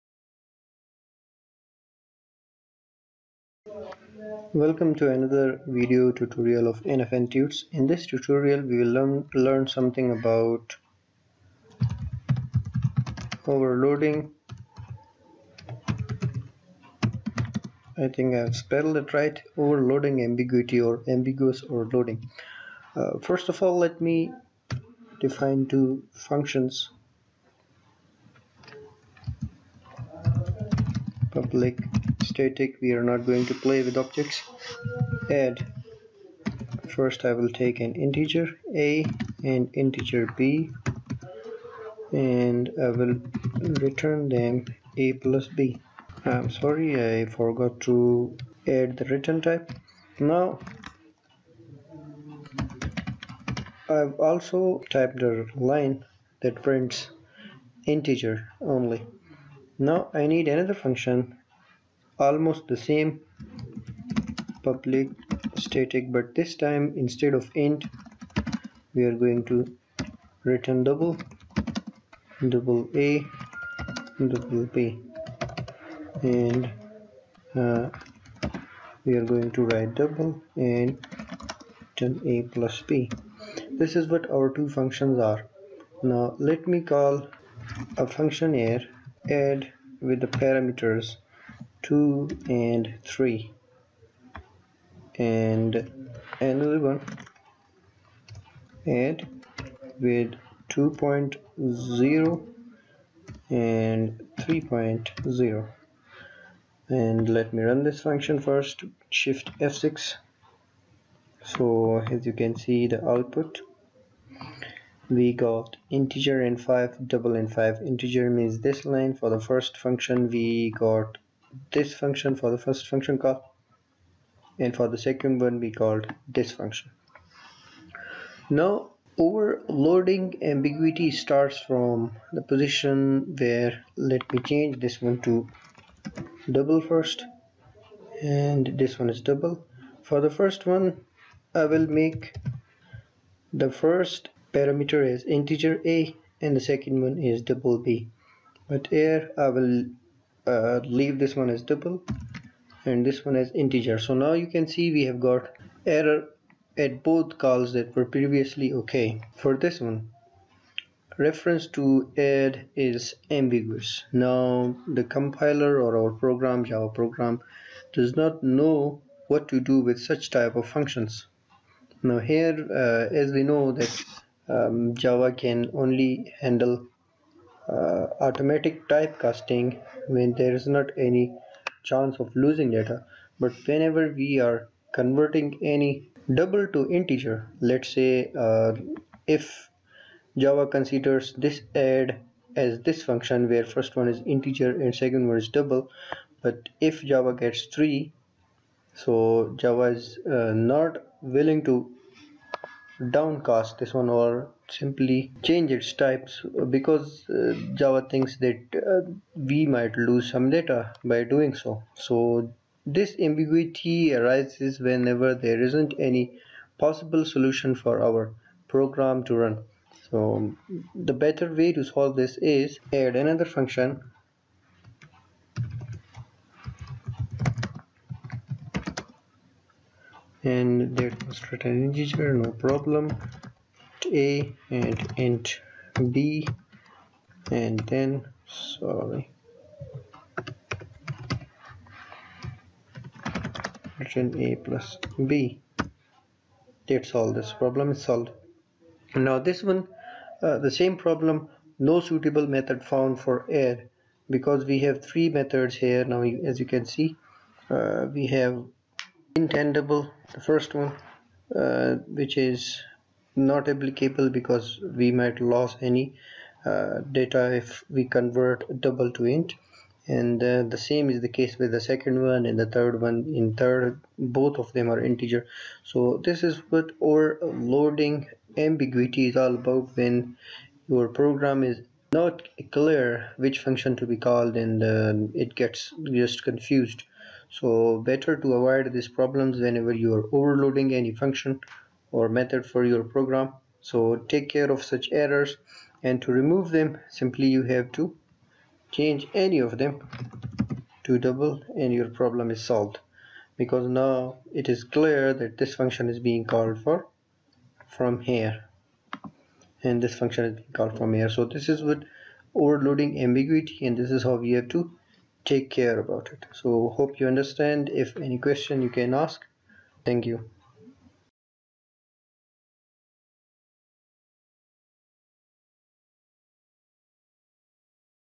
Overloading Ambiguity in java
this video demonstrate about the ambiguity of method overloading and will solve that problem